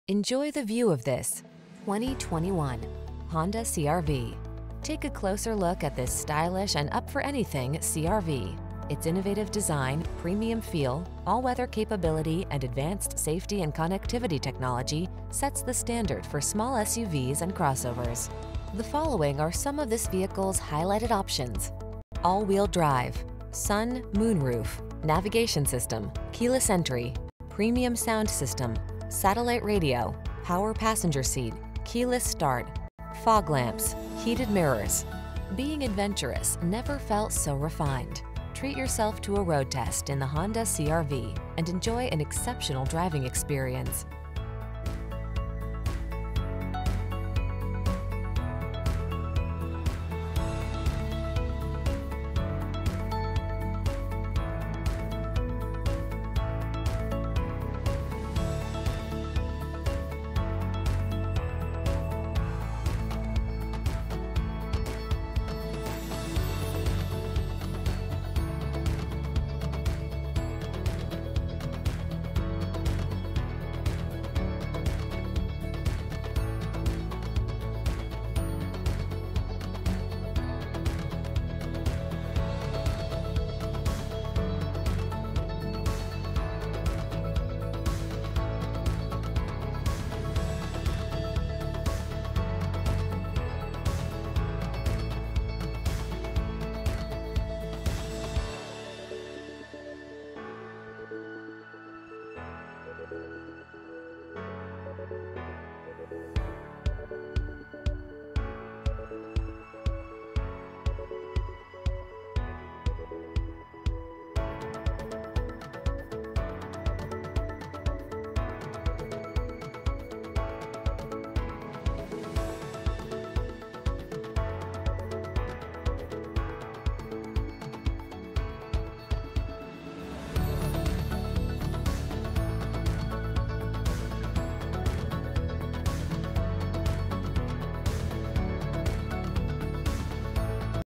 2021 Honda CR-V Aurora, Denver, Highland Ranch, Parker, Centennial, CO 46206
Modern Steel Metallic New 2021 Honda CR-V available in Aurora, Colorado at

Delivers 32 Highway MPG and 27 City MPG! This Honda CR-V delivers a Intercooled Turbo Regular Unleaded I-4 1.5 L/91 engine powering this Variable transmission. Wheels: 19 Machine-Finished Alloy w/Gray Inserts, Voice Activated Dual Zone Front Automatic Air Conditioning, Valet Function.* This Honda CR-V Features the Following Options *Trunk/Hatch Auto-Latch, Trip Computer, Transmission: Continuously Variable w/Sport Mode, Tires: 235/55R19 101H All-Season, Tailgate/Rear Door Lock Included w/Power Door Locks, Systems Monitor, Strut Front Suspension w/Coil Springs, Steel Spare Wheel, Speed Sensitive Rain Detecting Variable Intermittent Wipers, Sliding Front Center Armrest and Rear Center Armrest.* Visit Us Today *You've earned this- stop by Mile High Honda located at 2777 S Havana St, Aurora, CO 80014 to make this car yours today!